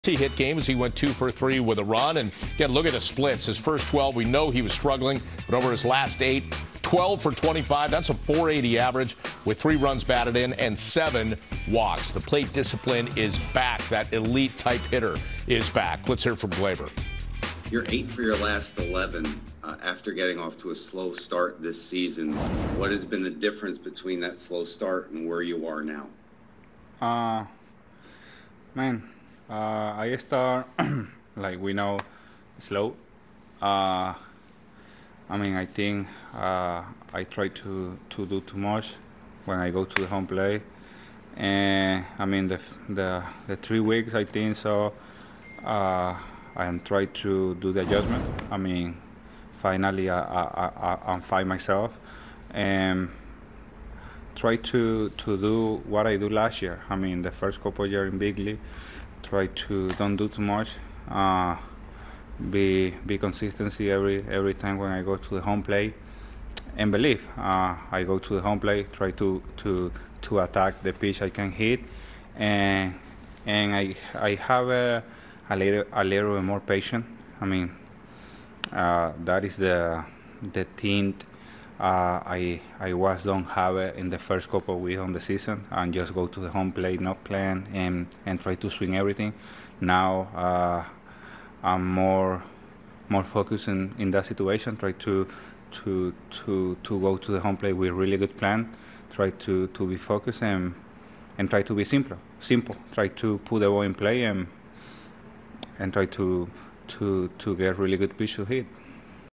Gleyber Torres has reached base safely in 8 straight games
Yankees infielder Gleyber Torres discusses how he is feeling at the plate. and the Yankees 4-2 win over the Red Sox.